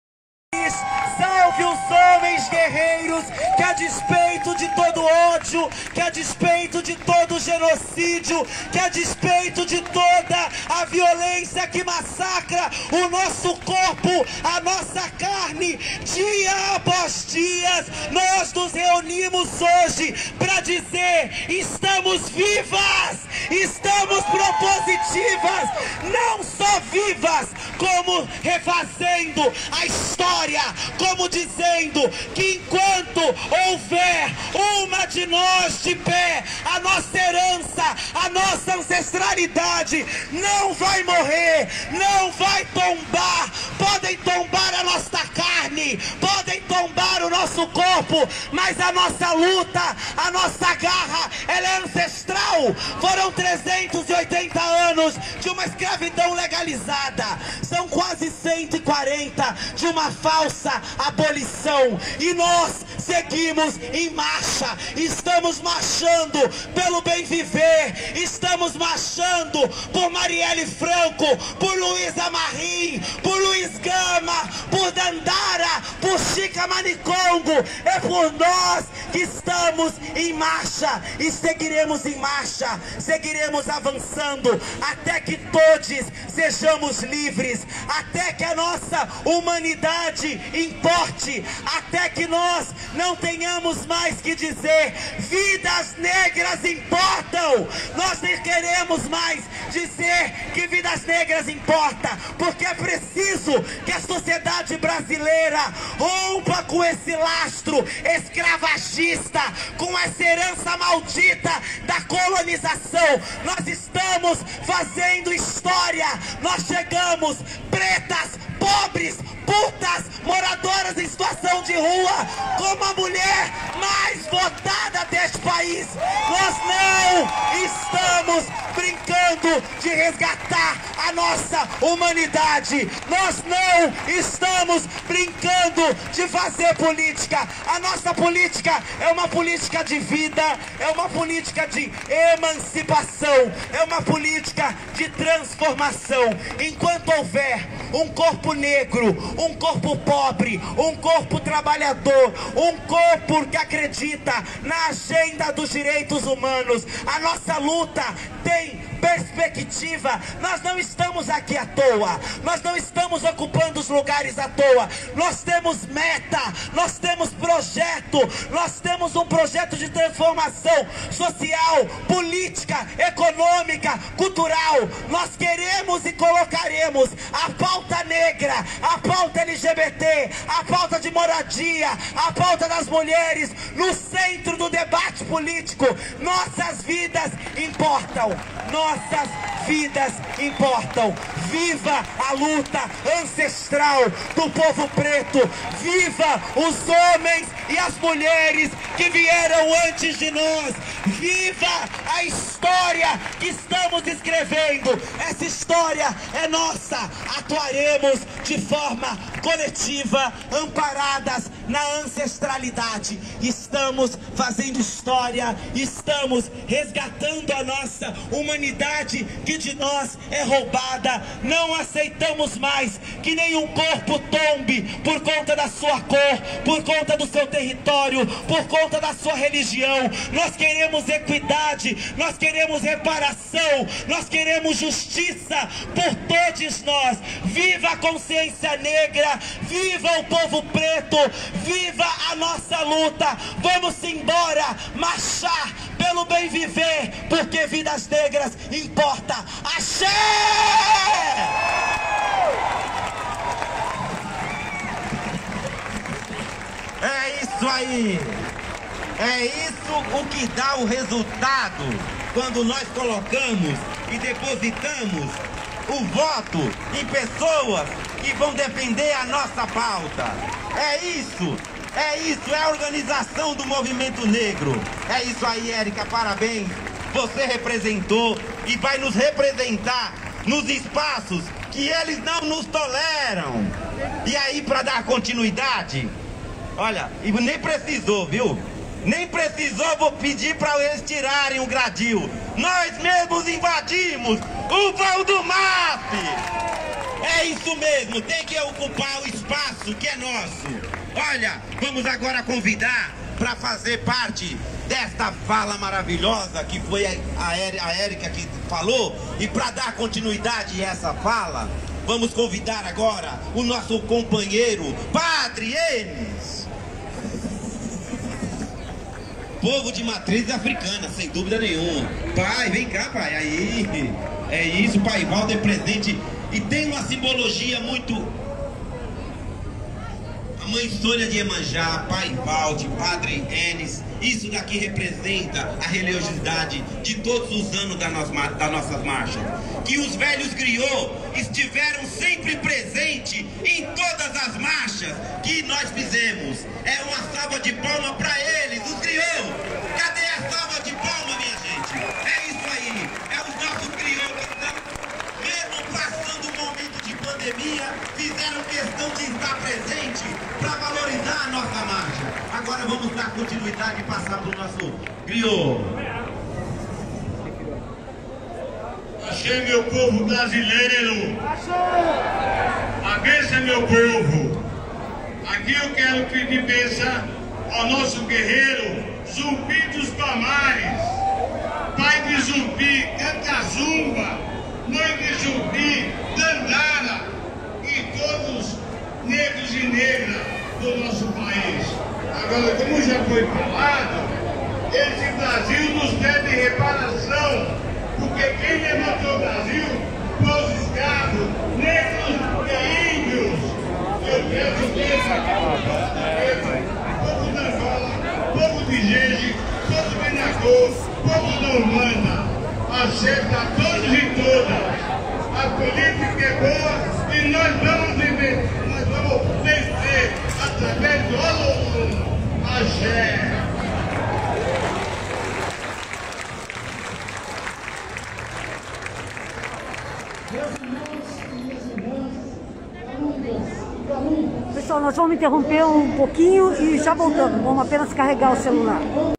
ATO DA 17ª MARCHA NEGRA EM SÃO PAULO-CONTINUAÇÃO
ATO DA 17ª MARCHA NEGRA EM SÃO PAULO
Ato contra a morte brutal de João Alberto Silveira Freitas no Carrefour do Rio Grande do Sul

AJUDE OS JORNALISTAS LIVRES A CHEGAR MAIS LONGE